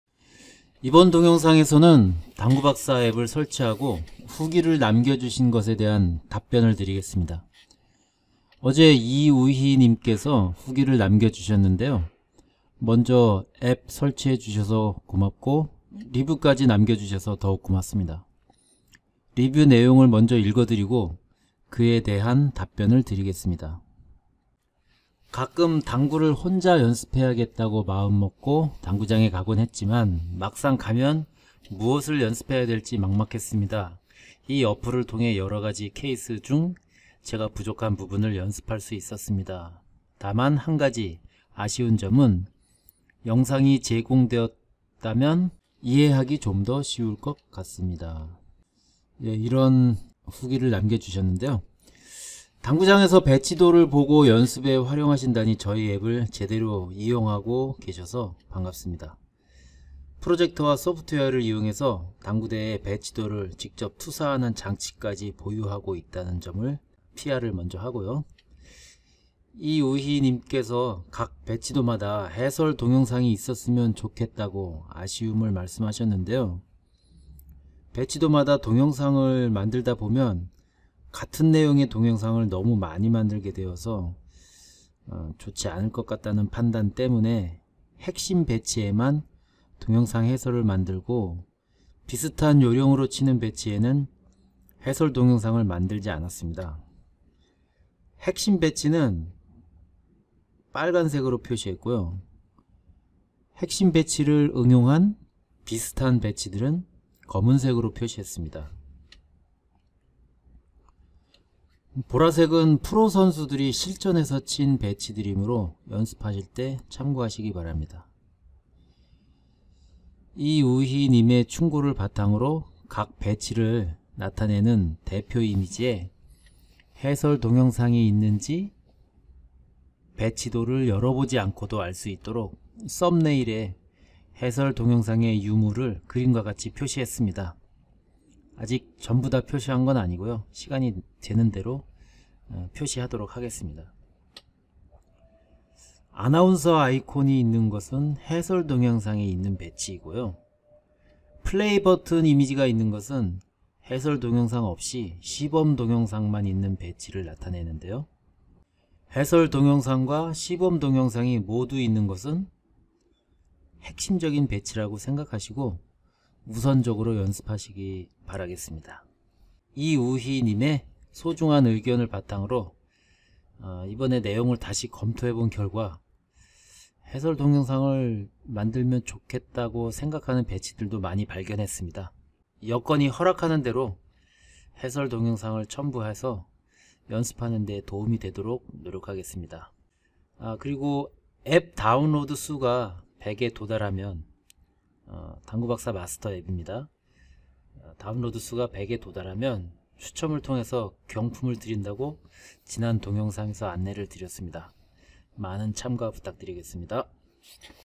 당구박사 당구레슨 마스터 어플 사용법 (리뷰에 대한 답변)
* 당구박사 당구레슨 마스터 어플에 리뷰를 남겨주신 것에 대한 답변입니다.
* 구글플레이스토어 에서 보기